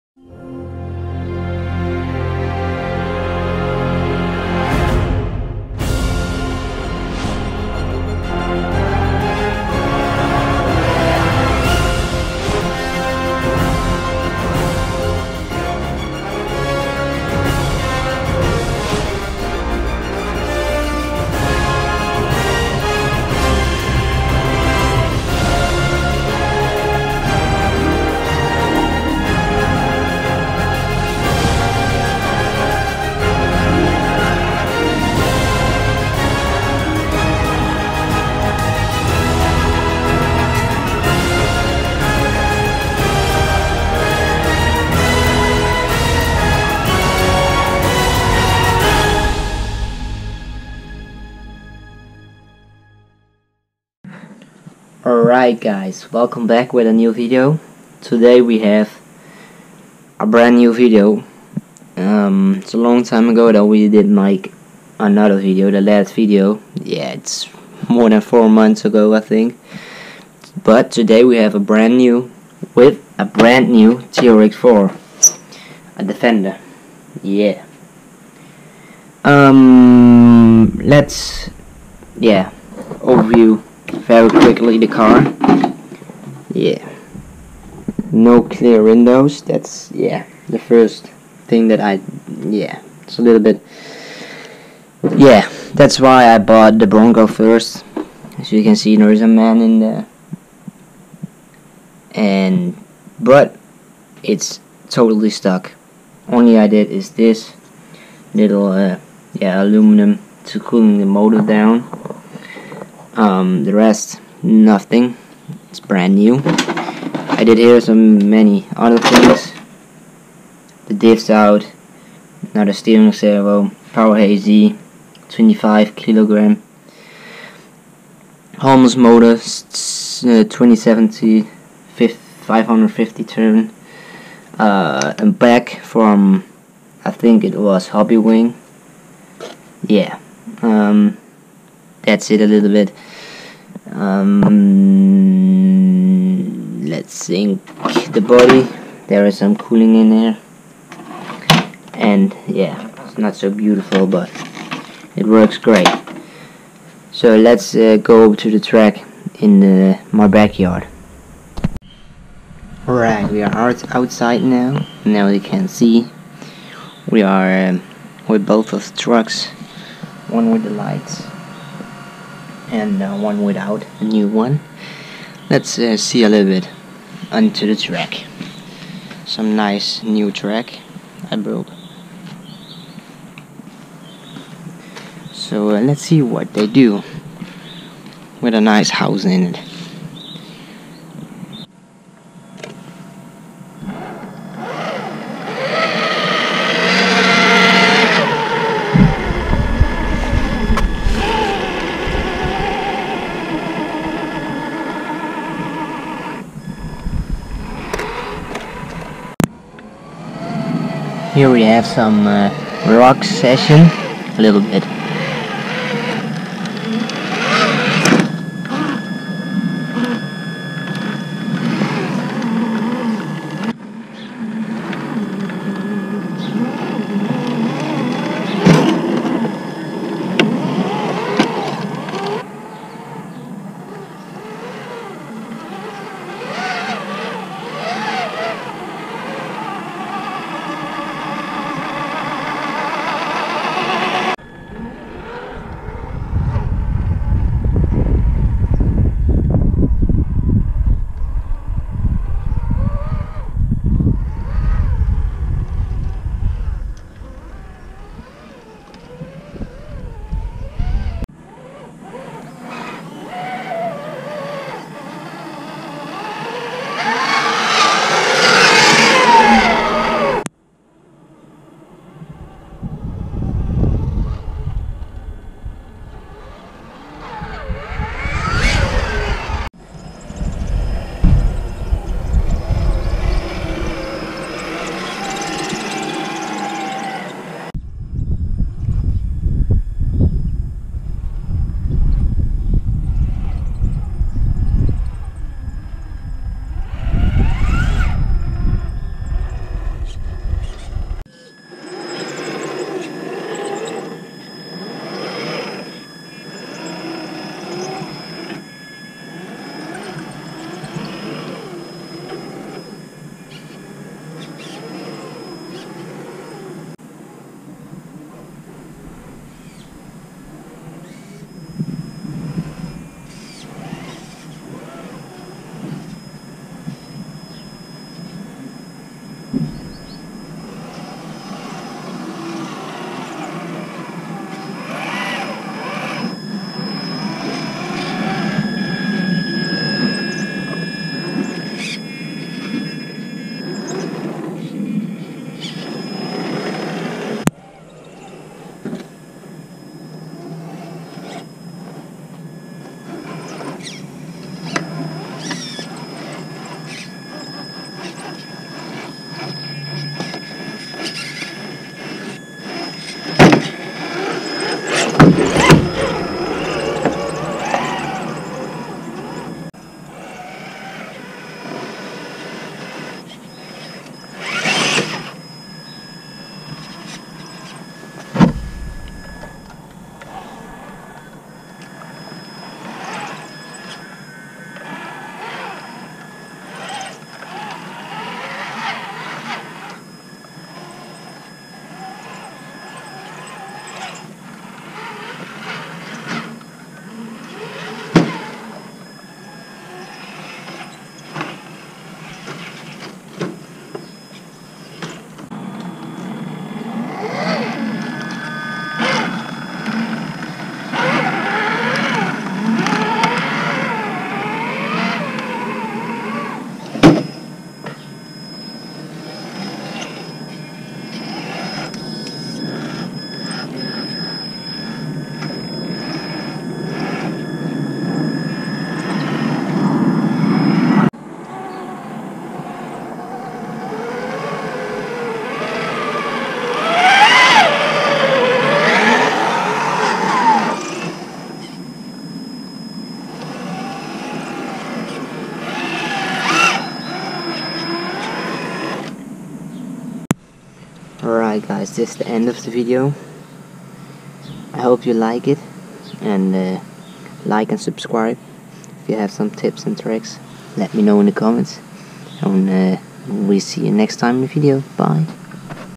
driving on my track in my backyard with my Traxxas bronco and the Traxxas defender
cars are driving on 3S
filmed with GoPro hero 5 session